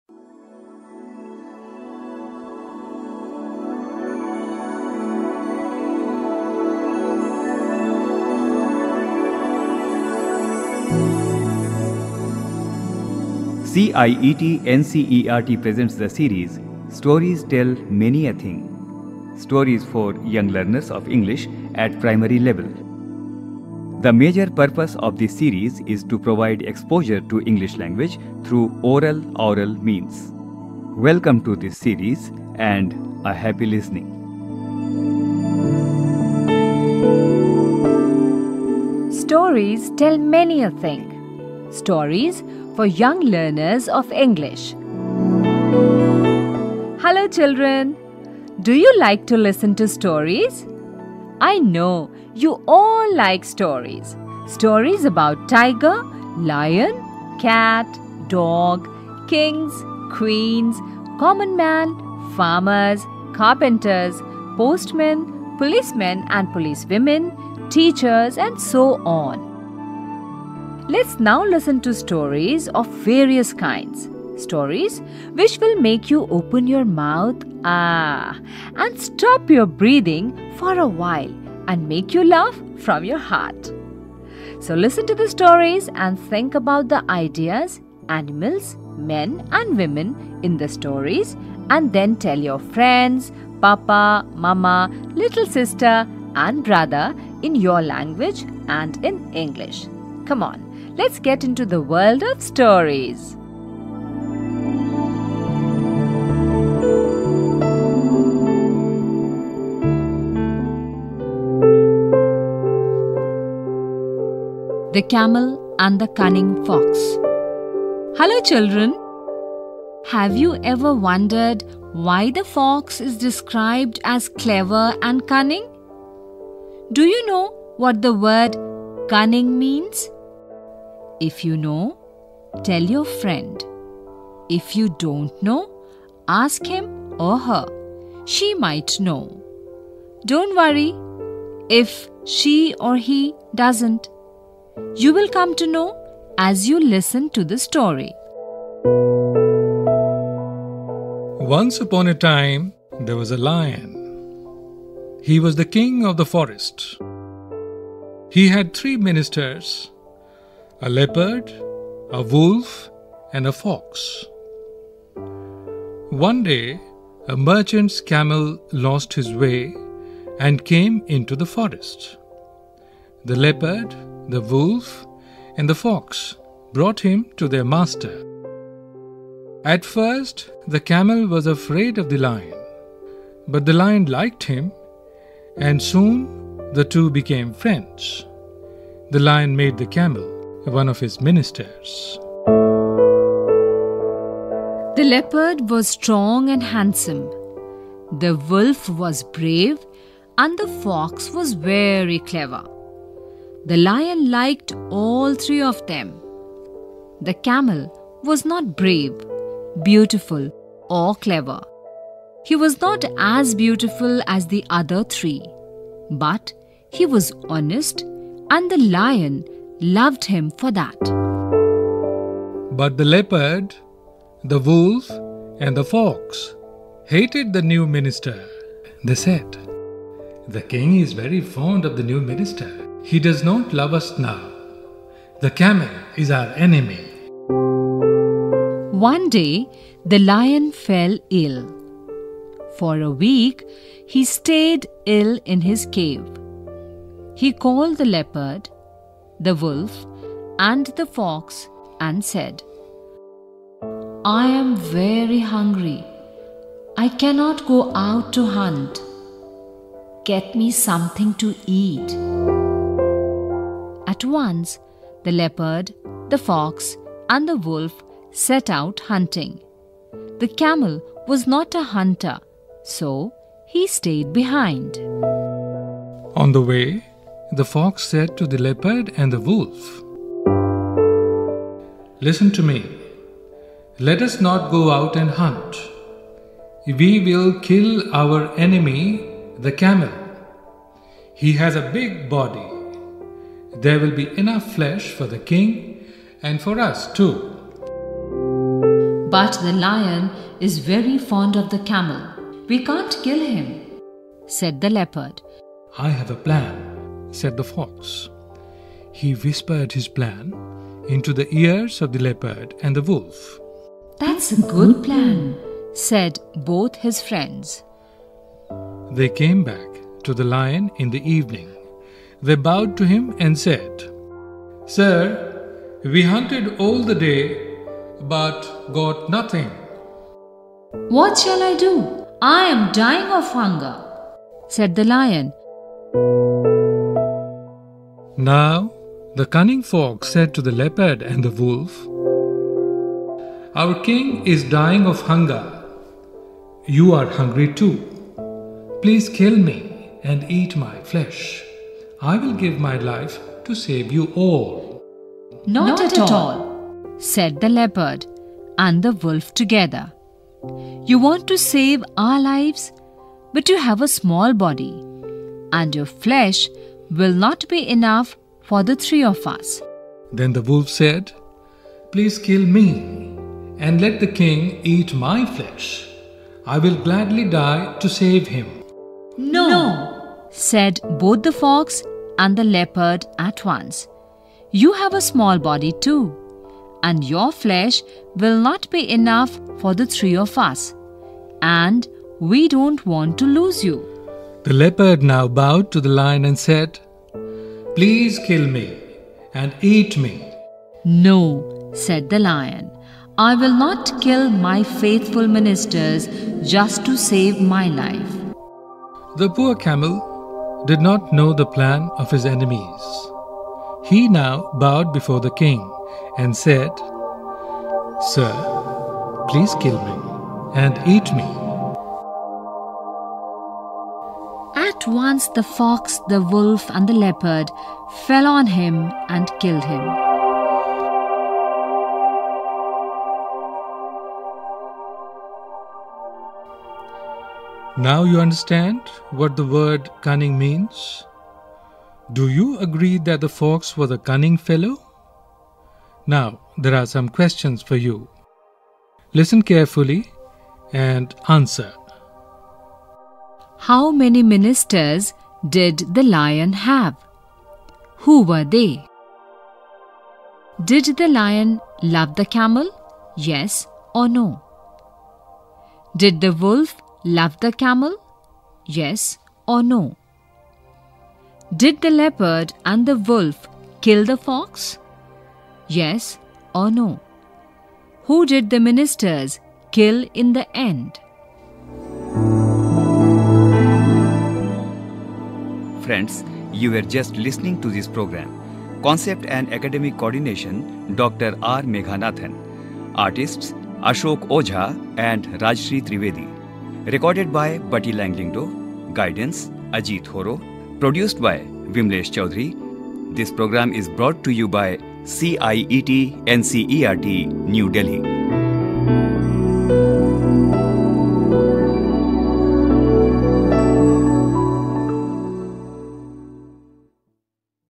The Camel And the Cunning Fox
This program  is a story of a camel and the fox. Because of the innocence and loyality the camel was killed by his friends.